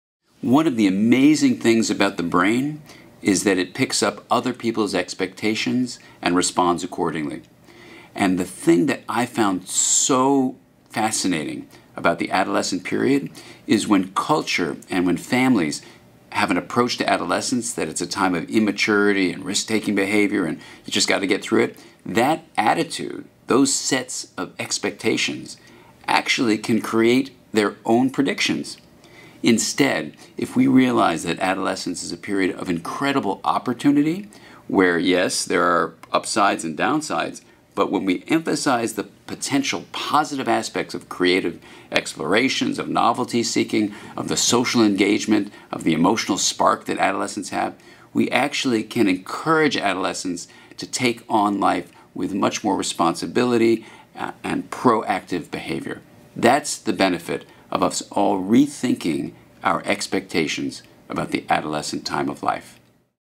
Dr. Dan Siegel - BRAINSTORM: Expectations and outcomes in adolescence
Dr. Dan Siegel discusses his latest book, Brainstorm.

Want to see more of Dan's videos?